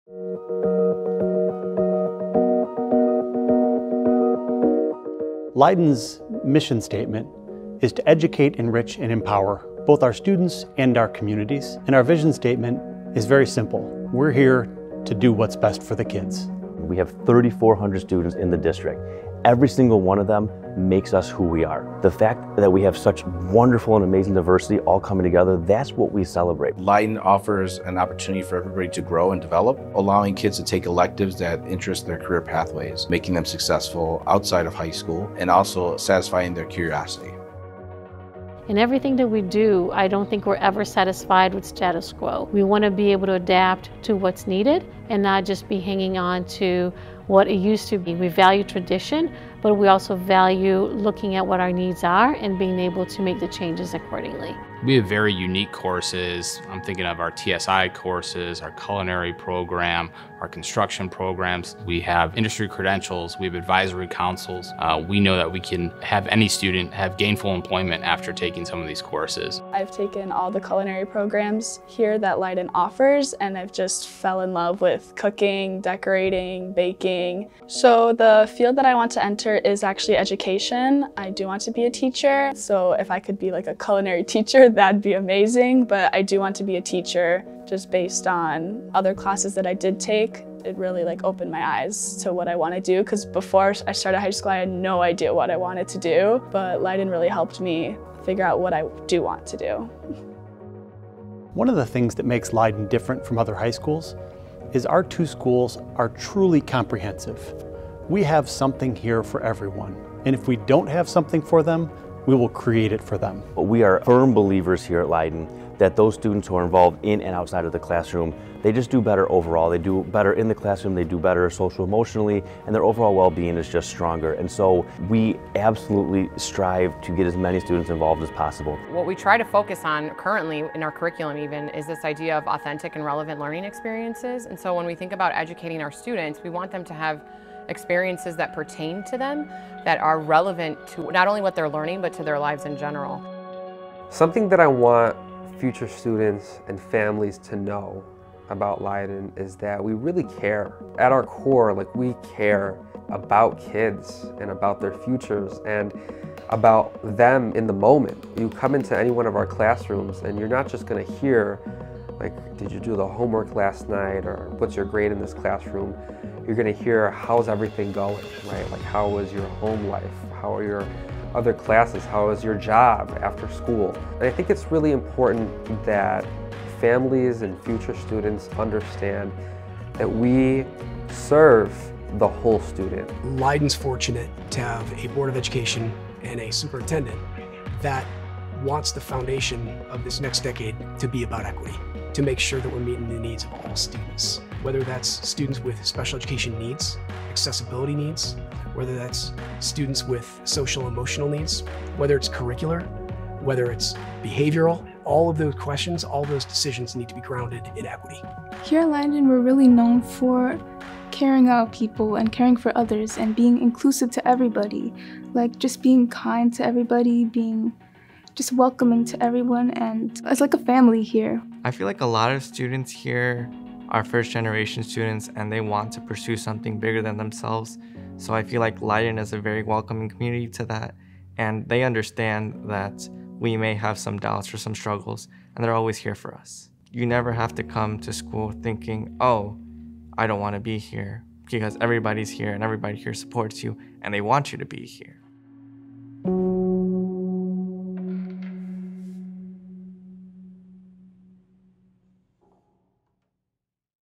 Leyden High School District 212 Overview Video
Leyden School District's mission is to educate, enrich, and empower its diverse community of 3,400 students through comprehensive academic programs, clubs, and sports. We aim to prepare students for future success with offerings from career pathways to AP courses. Leyden boasts a national reputation for innovation and has been recognized for equity programming, top high school rankings, and as a top workplace. Our recent renovations enhance our academic, athletic, and community offerings for generations to come.

Join us and soar with Leyden Eagles!